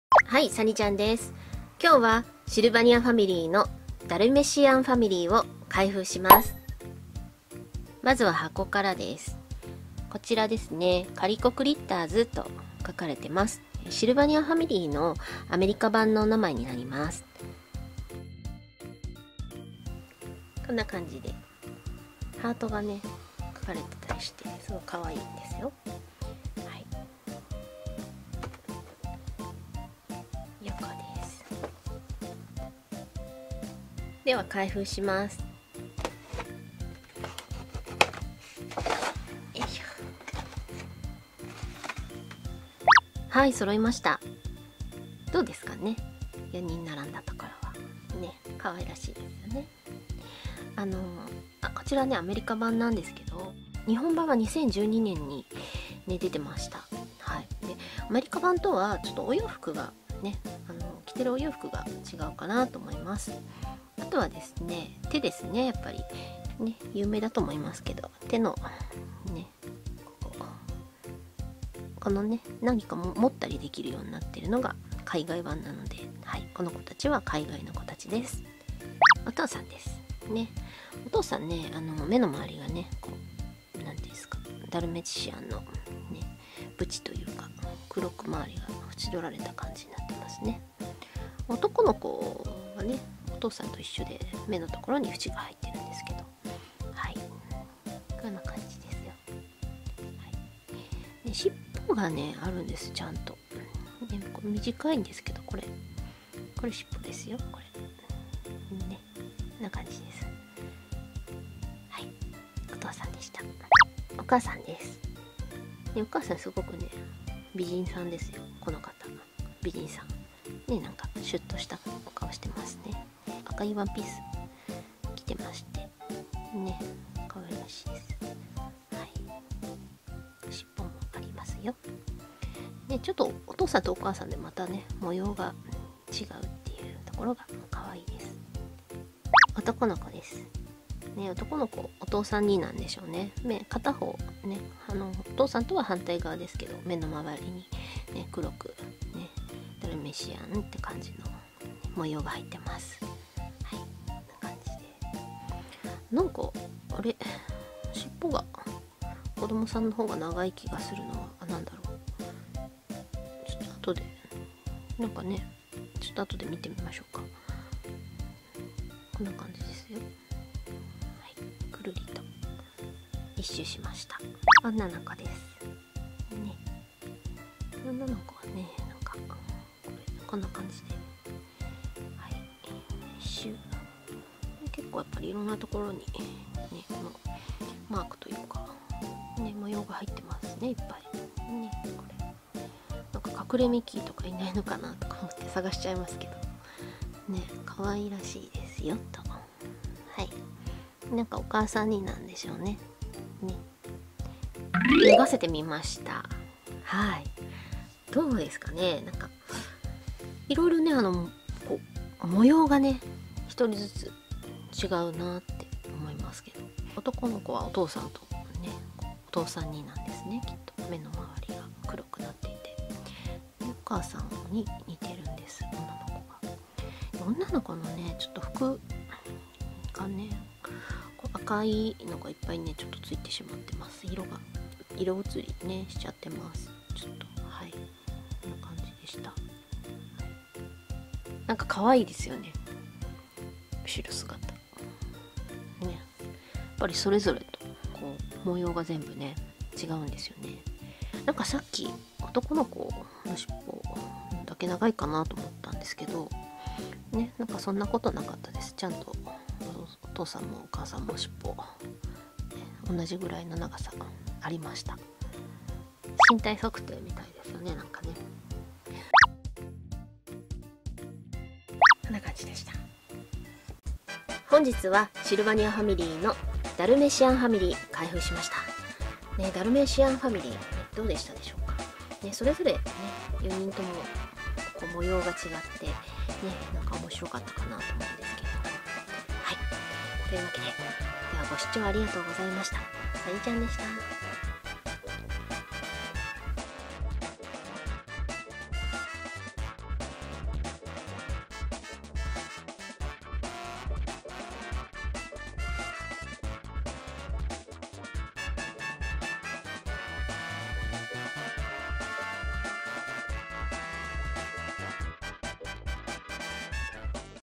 【開封】USダルメシアンファミリー・ Dalmatian family☆シルバニアファミリー☆Calico Critters【Sylvanian Families】
The Dalmatian family of the Sylvanian family has been opened.
US版シルバニアファミリー【ダルメシアンファミリー】の開封動画です。
 ぜひ、一緒に開封のドキドキわくわくを体験してください♥

※この動画は玩具好きの大人の為の動画です。